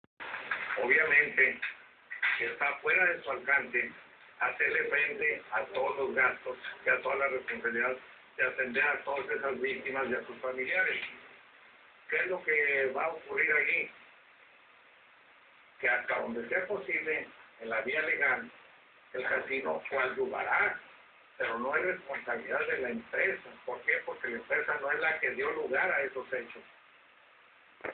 REPRESENTANTE DEL CASINO ROYAL NO SE HACE RESPONSABLE
.EL LIC. JUAN JOSE GOMEZ, REPRESENTANTE LEGAL DEL CASINO "ROYALE" DICE K ELLOS TAMBIEN FUERON -VICTIMAS- DEL -CRIMEN ORGANIZADO-K LE KISIERON COBRAR -PISO- AL CASINO LO CUAL SE NEGARON A DARLES Y POR LO TANTO LES INCENDIARON EL LOCAL PERO K ELLOS BENEFICIARAN SOLO A LOS EMPLEADOS DEL CASINO, MAS A LAS FAMILIAS DE LOS MUERTOS NO TIENEN LA OBLIGACION YA K EL CASINO TAMBIEN FUE PERJUDICADO Y EL INCENDIO FUE PROVOCADO POR AJENOS